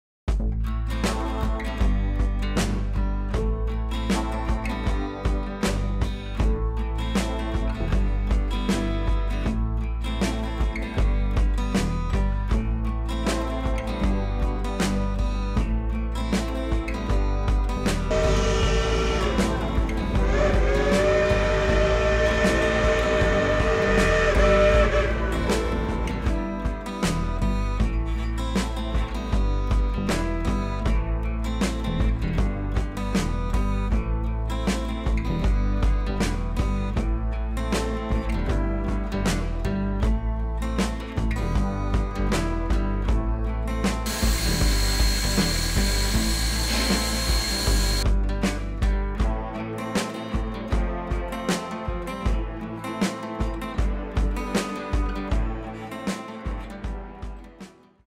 Durango Silverton Narrow Gauge Railroad
The Durango Silverton Narrow Gauge Railroad is a scenic, tourist, narrow gauge railroad that runs between Durango, Colorado and Silverton, Colorado. Scenes from this video were taken from our summer of 2022 road trip that we called 21 for 21.

Thinking about building your own website?